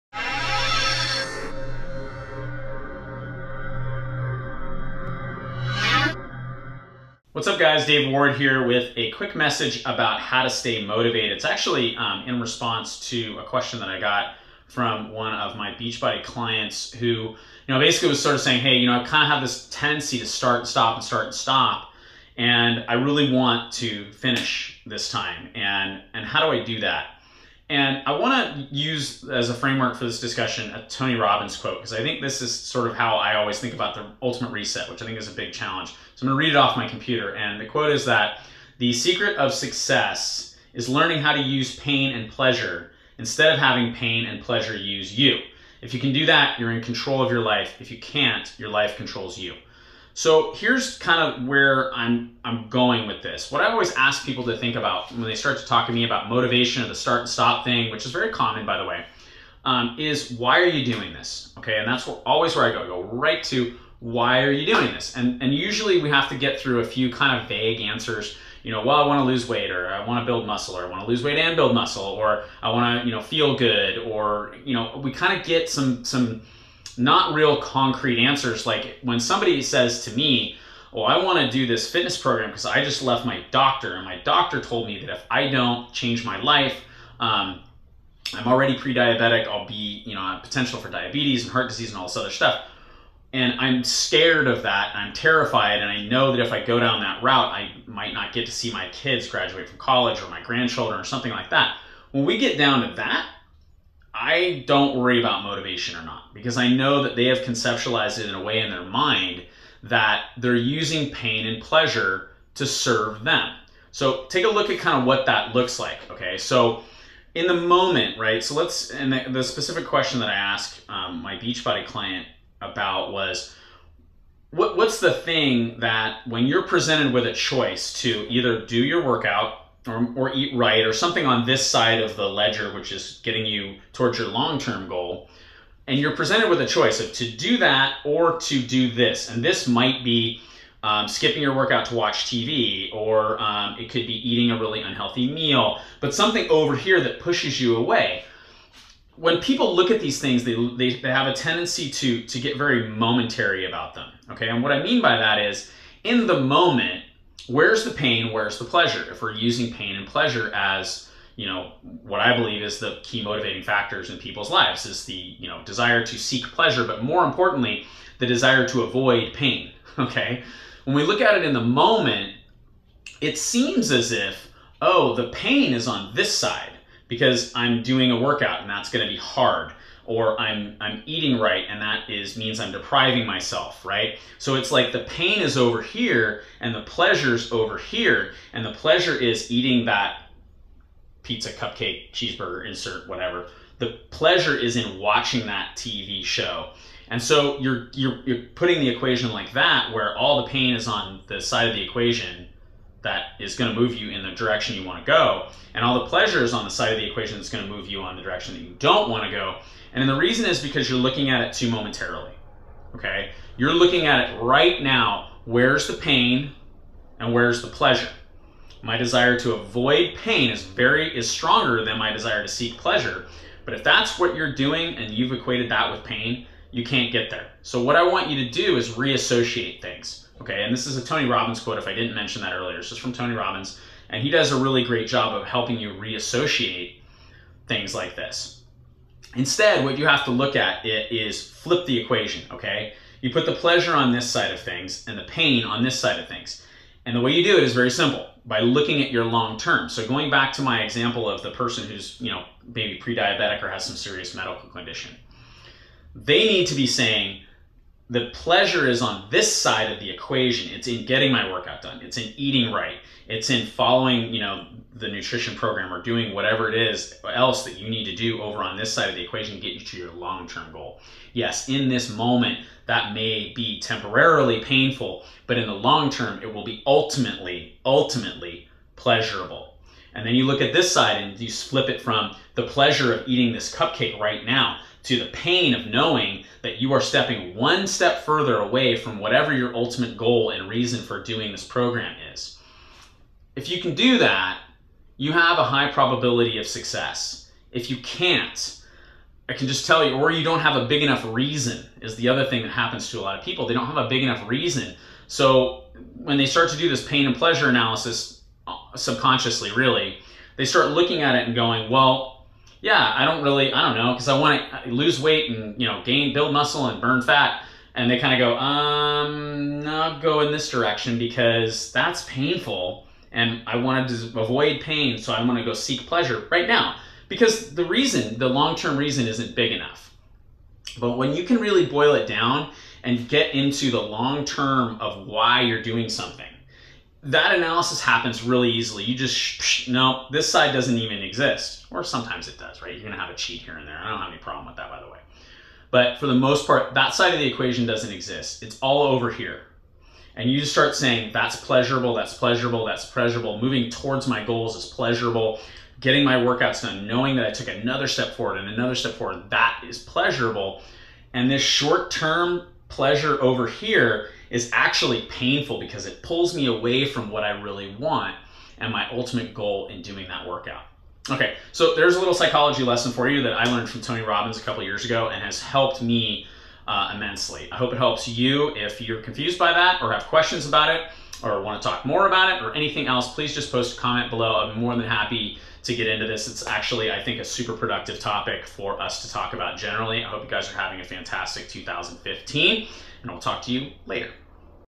How to Stay Motivated
Motivation is one of the key things that people look to me for when I Coach them - whether it be in fitness, nutrition or business. I wish I could serve it up on a China dish with a silver spoon. The truth is that I can't. Here is what YOU can do to find motivation where it truly belongs: WITHIN YOU.

If you're even remotely interested in doing what I do, start poking around on our website where we have a ton of info